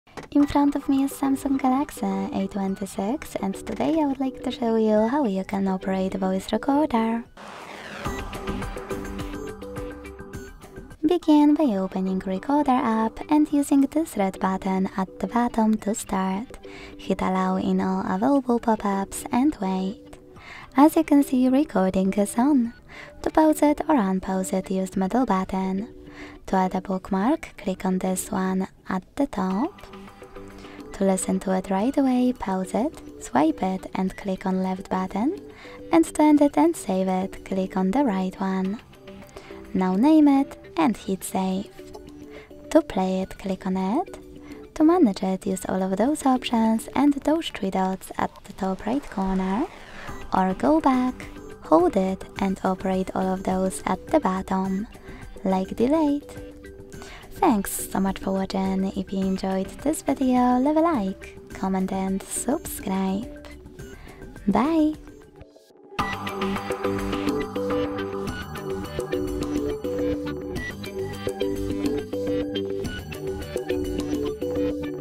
SAMSUNG Galaxy A26 5G – How to Record Audio with Voice Recorder App
Learn how to record sounds and manage audio files on your SAMSUNG Galaxy A26 5G using the built-in Voice Recorder app. This video will show you step-by-step how to start a new recording, pause and resume, add bookmarks, save and name your audio files, and play them back. You’ll also discover how to manage your recordings, including deleting or organizing them with the available options. Whether you want to capture voice notes, lectures, or any important audio, this guide will help you get the most out of the Voice Recorder app on your Samsung Galaxy A26 5G.

How to record audio on SAMSUNG Galaxy A26 5G with Voice Recorder app?
How to save and play back recordings on SAMSUNG Galaxy A26 5G?
Where to find and manage audio files on SAMSUNG Galaxy A26 5G?

0:00 Open Voice Recorder app
0:08 Start a new recording
0:18 Allow permissions
0:22 Pause, resume, and add bookmarks
0:31 Save and name your recording
0:43 Play back audio files
0:50 Manage and delete recordings